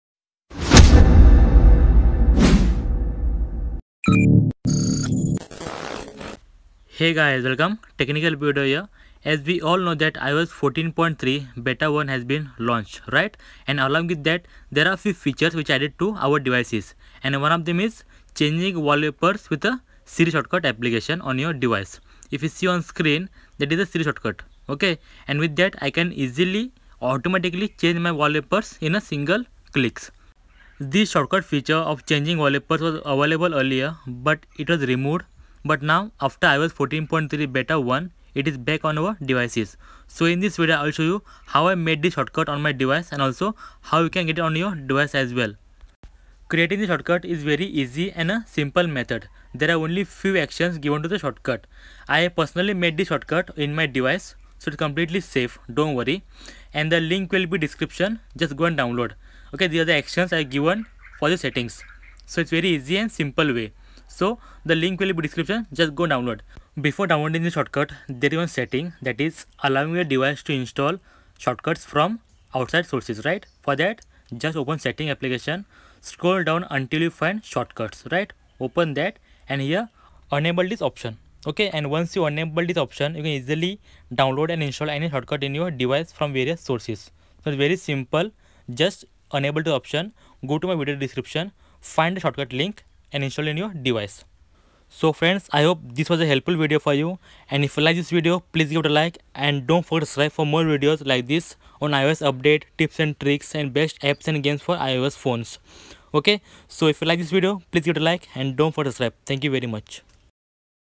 How To Set Wallpaper with Shortcuts app on iPhone I iOS 14.3 Beta 1 I iOS Shortcut for Set Wallpaper
How To Set Wallpaper with Siri Shortcuts app on iPhone I iOS 14.3 Beta 1 Wallpaper Shortcut Feature,  iOS Shortcut for Set Wallpaper

About Video:
iOS 14.3 Beta 1 has been released with many new features. One of them is setting automatic wallpapers on iPhone with Siri Shortcuts app.
iOS 14.3 Beta 1 Siri Shortcut feature where you can set automatic wallpaper is available now.
In this video learn how to set wallpapers with shortcuts app on iOS 14.3 Beta 1.
Shortcuts for setting wallpaper on the iPhone.
Set wallpaper shortcut on iPhone, how to use set wallpaper shortcut on the iPhone.